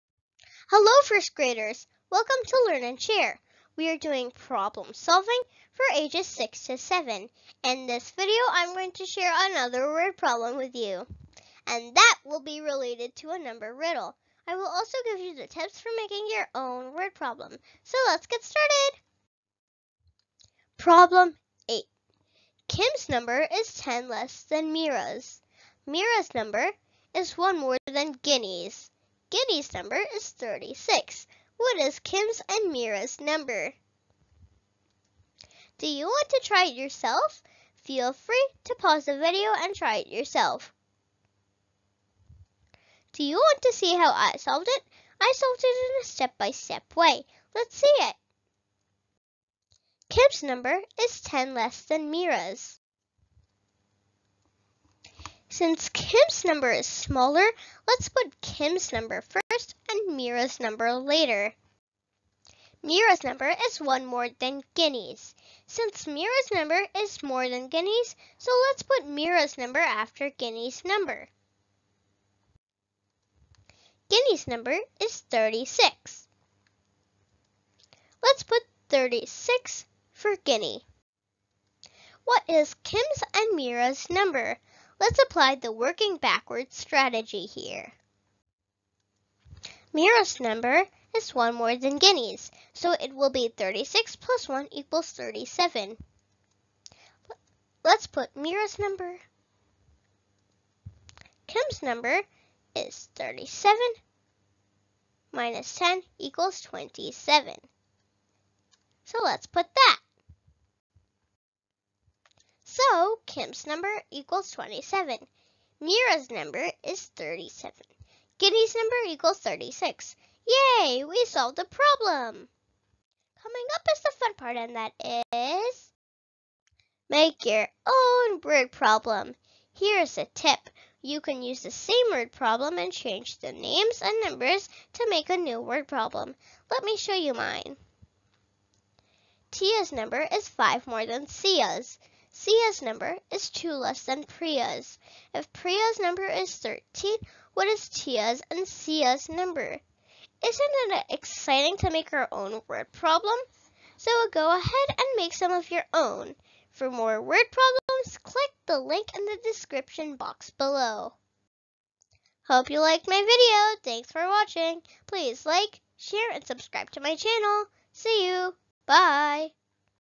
Finding Kim, Meera and Ginny's number in a Number Riddle Word Problem | How I solved a Number Riddle
Kim, Meera and Ginny had a certain number. We are given Ginny's number and some clues about Kim and Meera's number. We have to find those numbers.

This Word Problem is related to a number riddle. You can see how I solved the problem in my first grade. This video will help you to make your own Word Problem.

Enjoy Problem Solving!